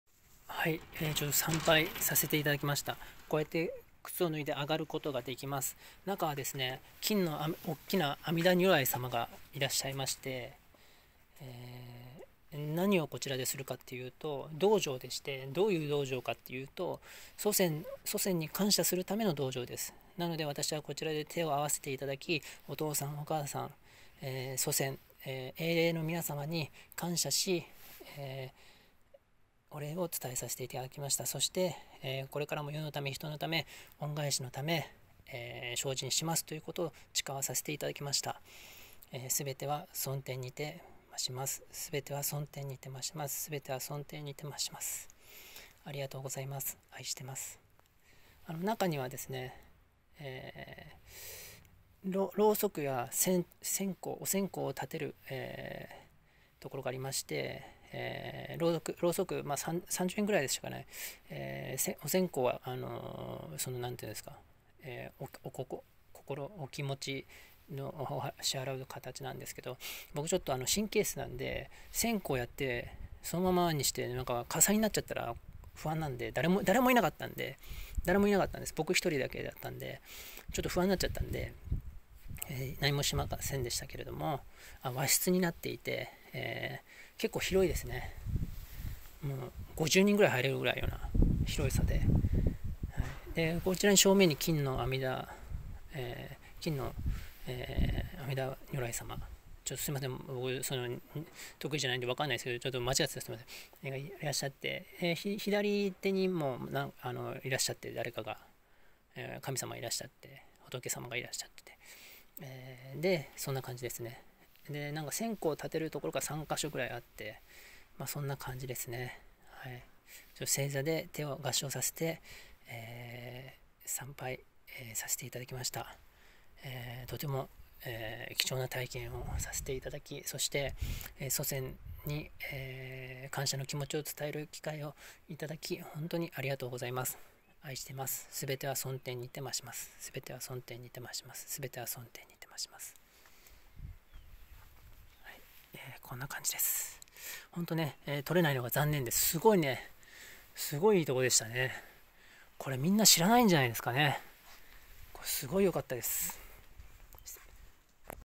第27回「鞍馬寺、転法輪堂の瞑想道場、感想」祖先に感謝するための道場です。
後藤輝樹の【鞍馬山（鞍馬寺）＆貴船神社】パワースポットツアーです。

鞍馬山は、
650万年前、サナトクマラ（護法魔王尊）が金星から地球に降臨した地であり、
98年前、臼井先生が決死の覚悟で鞍馬山に籠った結果、
21日後に臼井先生に靈氣(レイキ)の力が発現した聖地であります。

今回、入山させていただきまして、
鞍馬山が被災している現実を目撃しました。

皇暦2680年4月3日現在、
聖地鞍馬山の被災した現状を伝えます。

いち早く元氣になる事を願っています。

動画の最初のほう、
被災した現状をよく知らず、脳天氣なこと言っててすみません。。

ここに来るの最初で最期かもしれないし、
わざわざ横浜から京都まで来た記念として、
特に目的意識もなく動画を撮りながら入山してましたが、
今思うと、撮ってて良かったと思っています。
上げれたら上げようかなレベルだったので、雑な撮り方ですみません。。

鞍馬山、鞍馬寺、貴船神社に1人で訪れる際の参考になれば幸いです。

本当は団体ツアーで行く予定がコロナで中止になり、
しょうがないので1人で予定を立て、
4月8日の満月の時、
スーパームーンの日に行く事にして楽しみにしていましたが、
4月1日寝る前、突如「4月3日に行く」が頭に降りて来て、
何故か、翌日4月2日、夜行バスに乗っていました。
4月8日に行けなかったの残念でしたが、
この現状を伝えろという意志が働いたのだと思います。
自分でもビックリしています。

コロナも含め、
この世の幸せを祈願して来ました。

月のように美しく(慈愛)、
太陽のように暖かく(光明)、
大地のように力強く(活力)、すべては尊天にてまします。

新たな言靈を学びました。
有り難うございます。感謝です。愛してます。